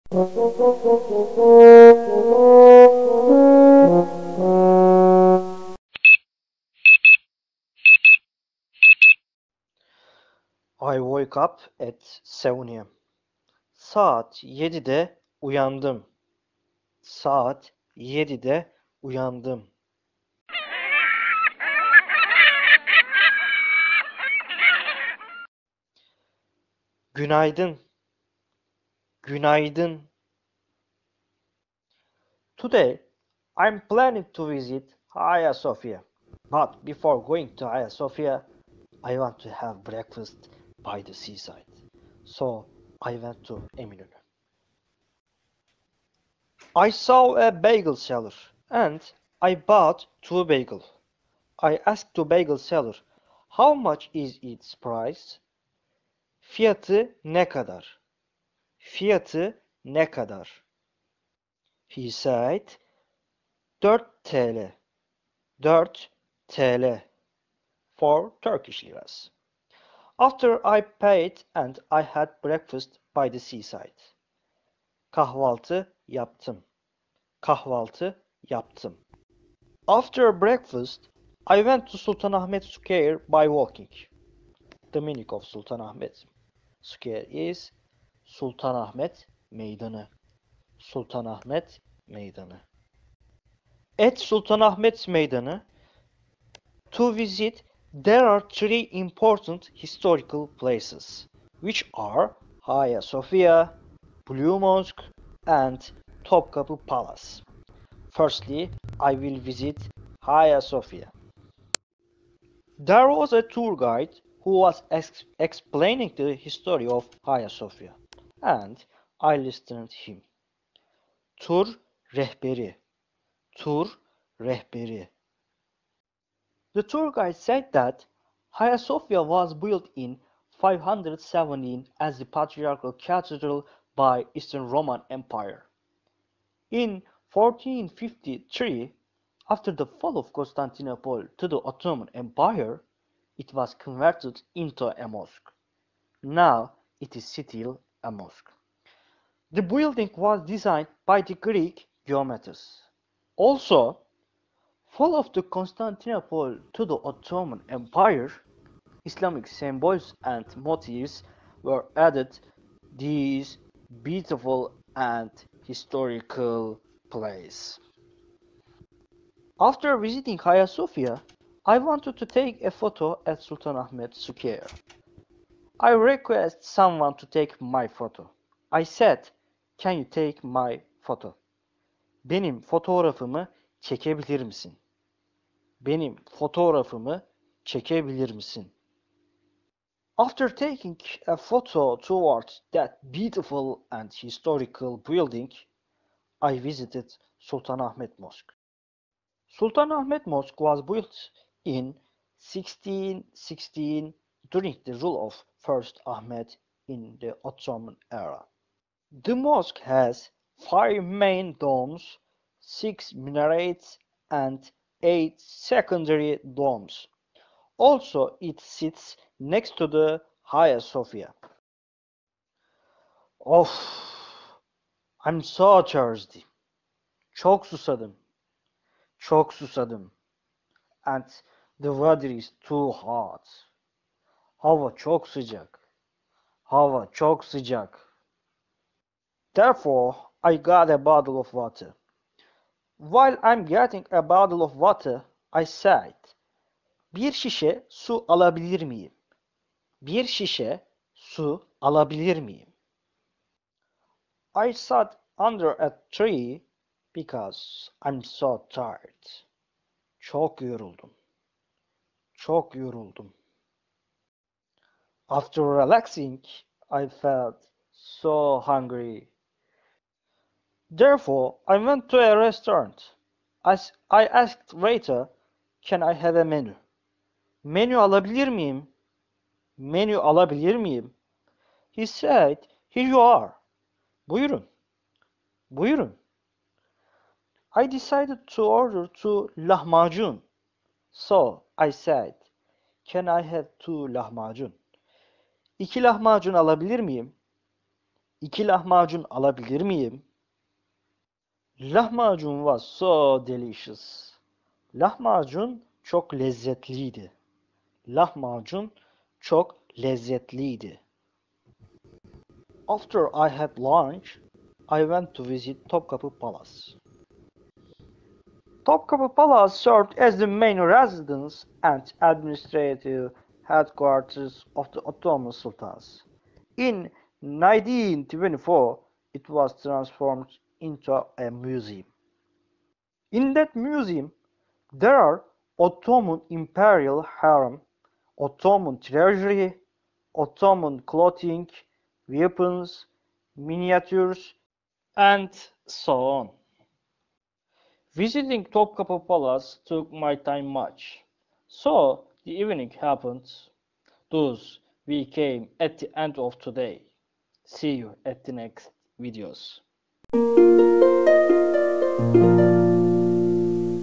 ISTANBUL
In that video, I tell some daily Turkish sentences and also about Istanbul. Therefore, while watching that video, you learn both Turkish and history of Istanbul.